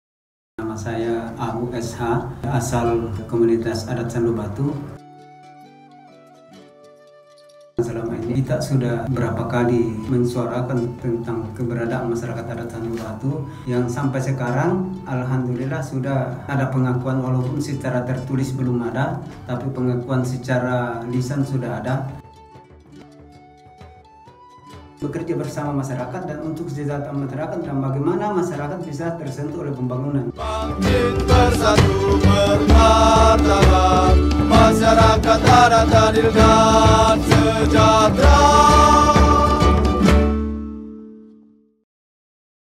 ABU, SH
Abu, perutusan politik Masyarakat Adat di Kabupaten Sidrap. Ia diutus maju sebagai Calon Anggota DPRD Kabupaten - Dapil III Kabupaten Sidrap, melalui Partai Kebangkitan Bangsa (PKB) nomor urut 4 (empat).